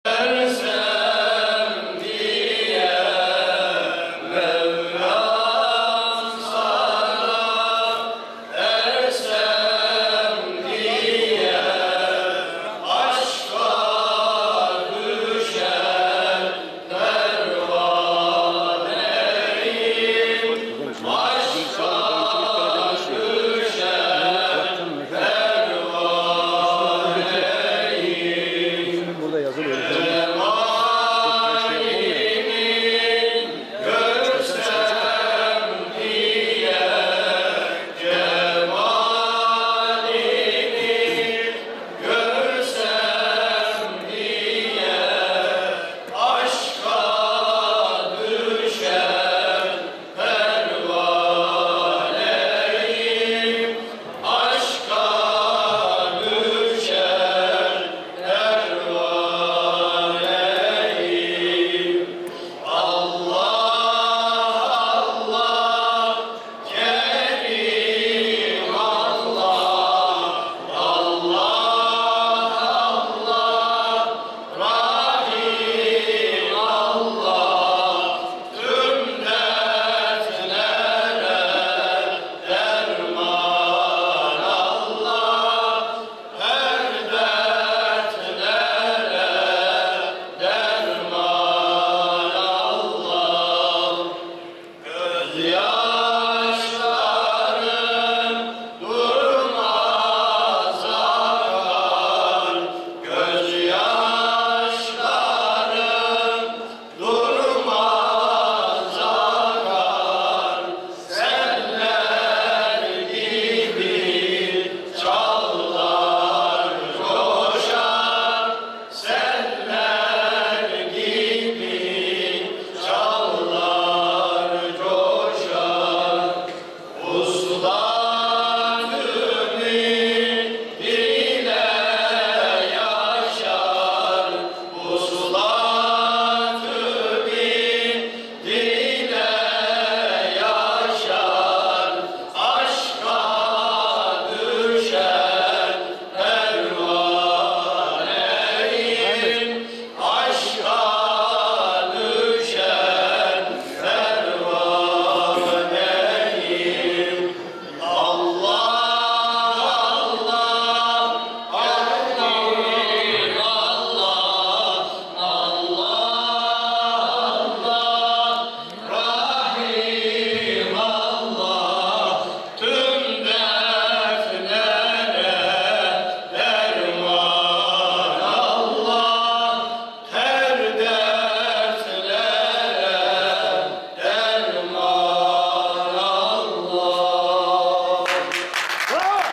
33. DÖNEM DİYANET EĞİTİM SEMİNERİ İLAHİ GURUBU VEDA PROGRAMI
ANTALYA MANAVGAT 33.DÖNEM EĞİTİM SEMİNERİ VEDA PROGRAMI